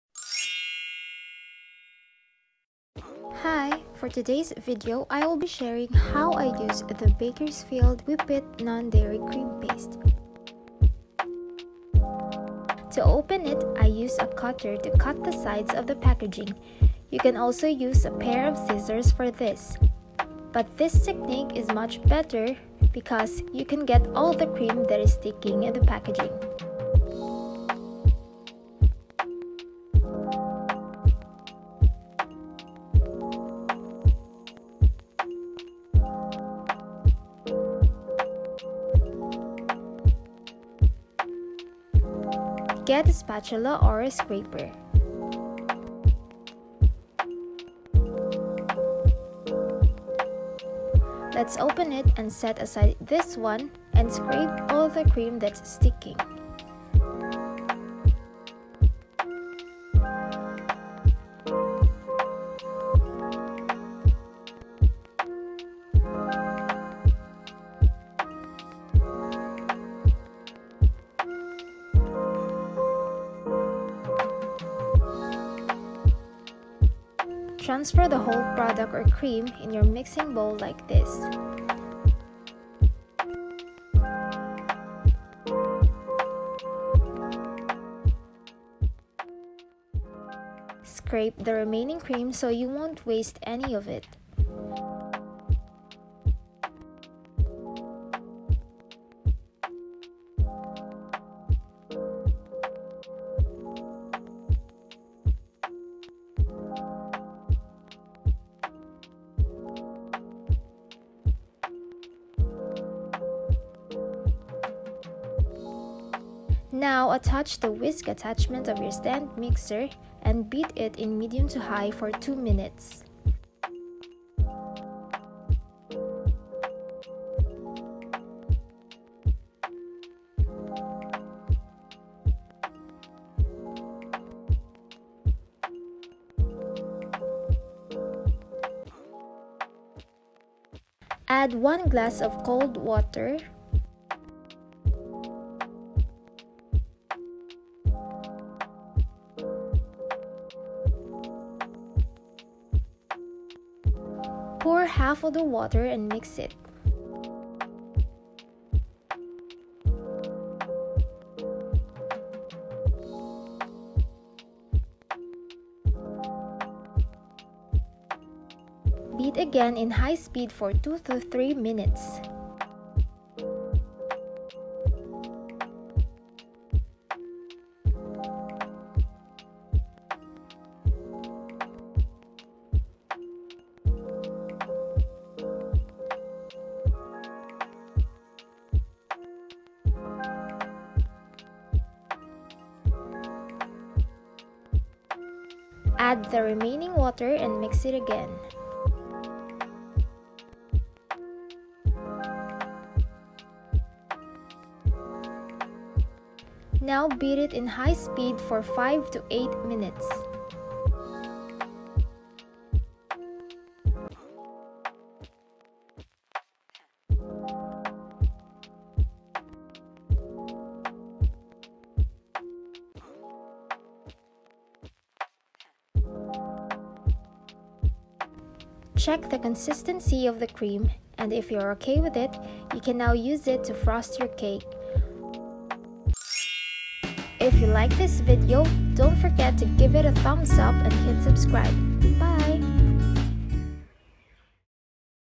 Whippit Frosting for Minimalist / Korean Cake | Easy and fast way to do it
Hey there! I’d like to thank everyone who supported me and push me to make a new video. You guys are asking what frosting I use to ice my cakes in mu previous videos, so here’s the answer!

What you need:
Bakersfield Whippit Non-diary Cream Paste1kg
1 glass of water or milk